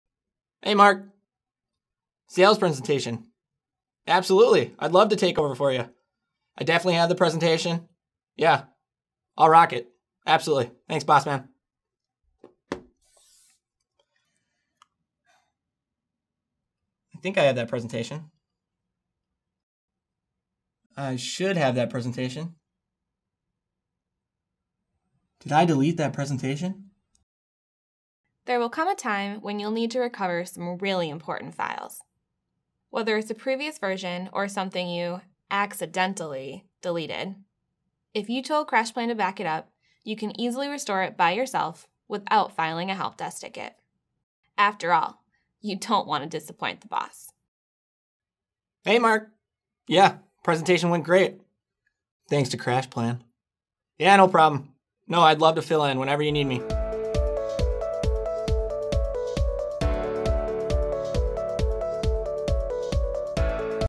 Deleted Files
Have you ever gotten a call from your boss asking for a file you just deleted?

With CrashPlan, recovering the file just takes a couple clicks.